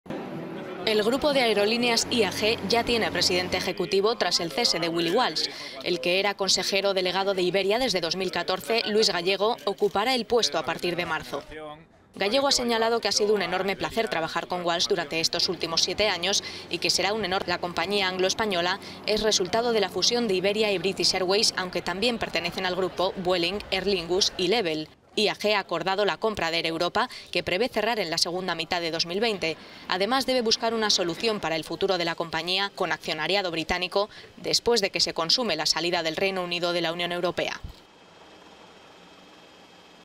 El presidente ejecutivo de Iberia Luis Gallego pasará a dirigir el grupo de aerolíneas IAG
La compañía anglo-española es resultado de la fusión de Iberia y British Airways y prevé cerrar la compra de Air Europa en 2020…